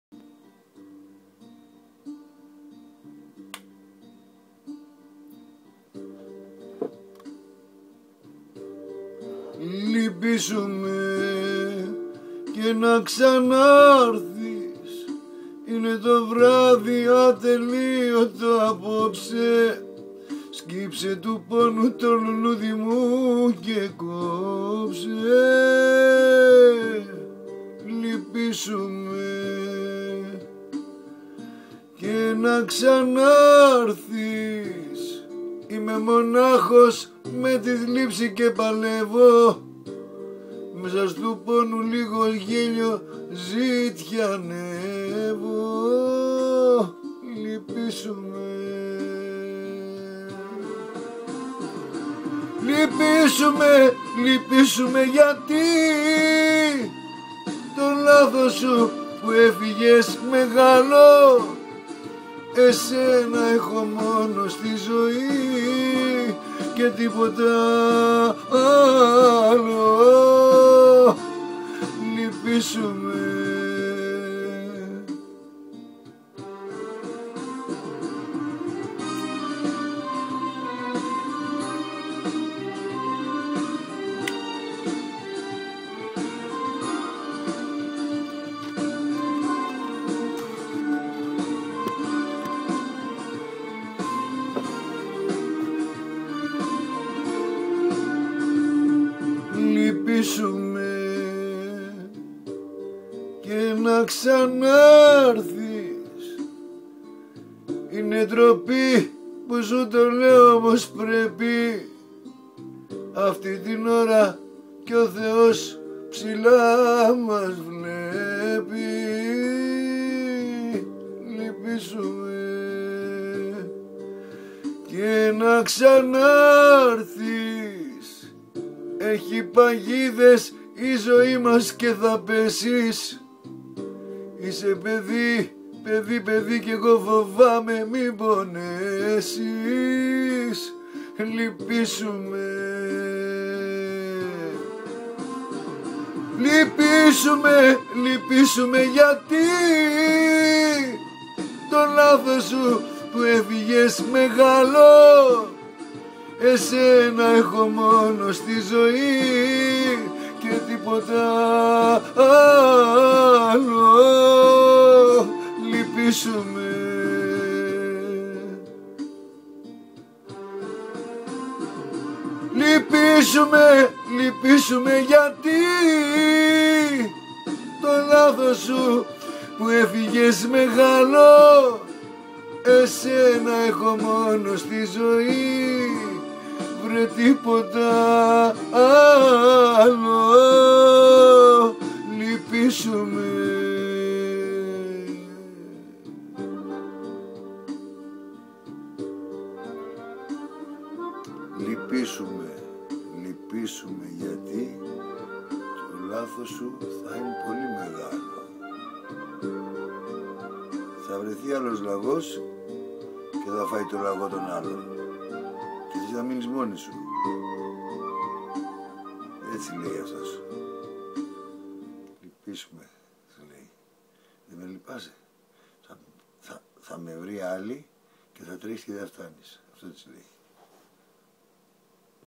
040{{{+kai na ksanarthis +}}}
o sotos prospathi na zografisei